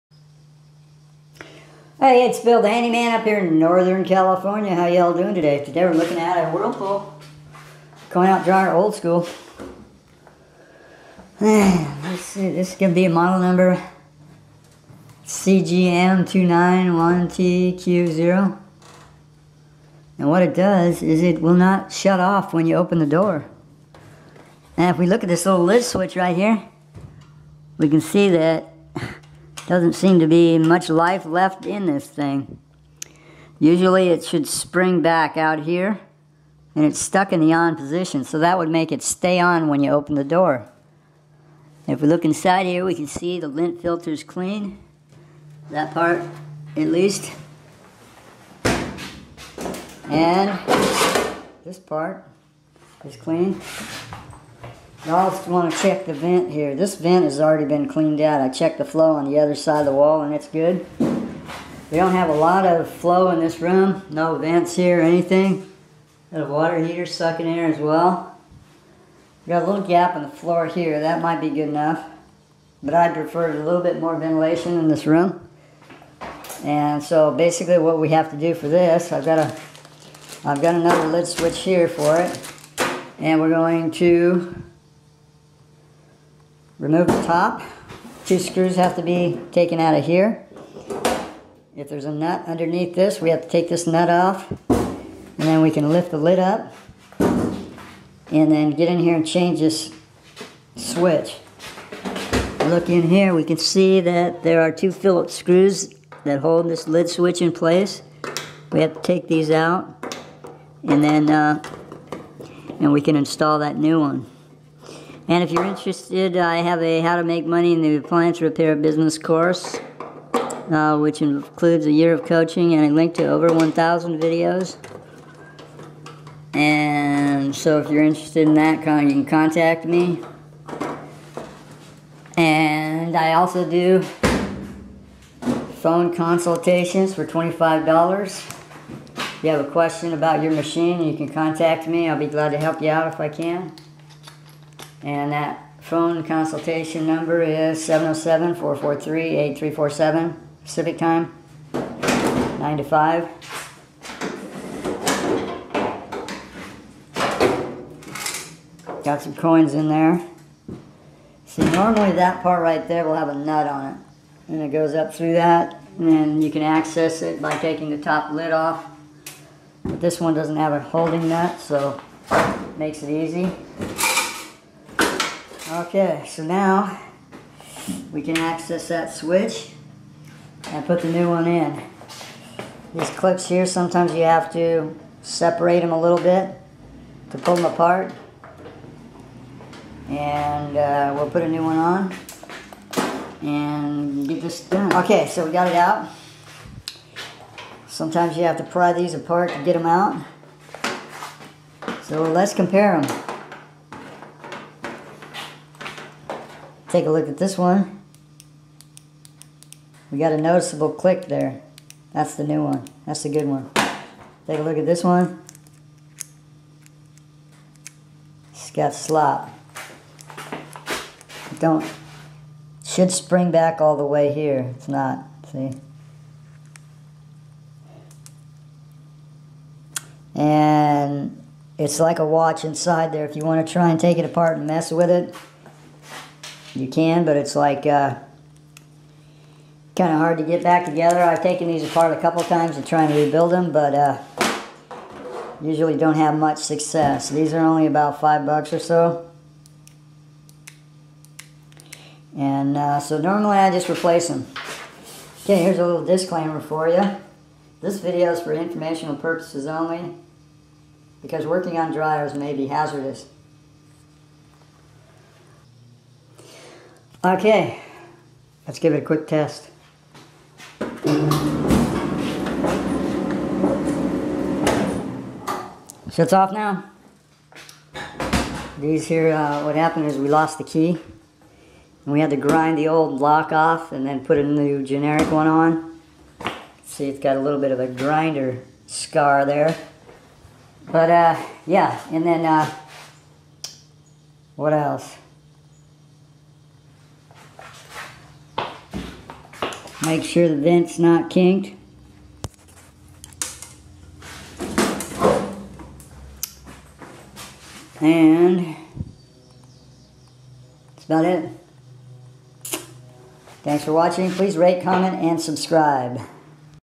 dryer wont turn off
Door switch's fall on or off. If it fails in the off position your dryer won't turn off. if it fails in the on position it will not turn off when you open the door.